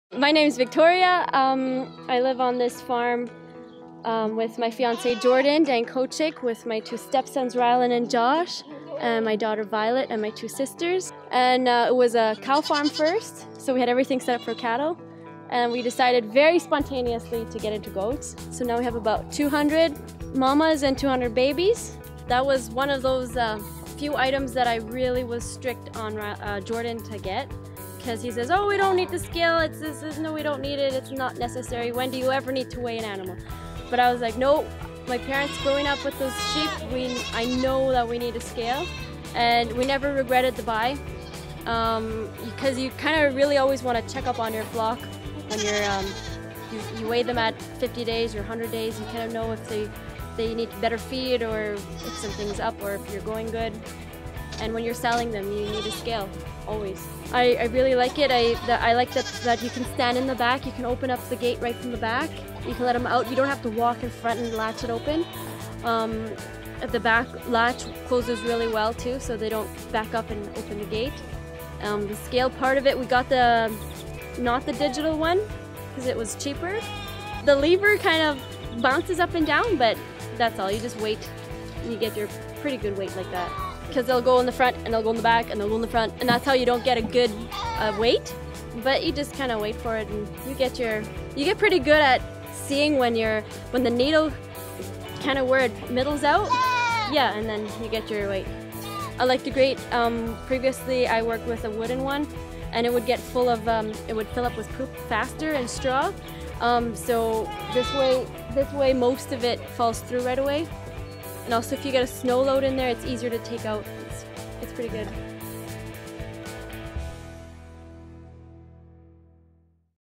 How a Lakeland Weigh Scale can help your sheep or goat farm
Have a listen to Victoria from Whiskey Road Farm, and how she and her goat farm make use of Lakeland's Weigh Scale! It's definitely a must have for any sheep or goat farm.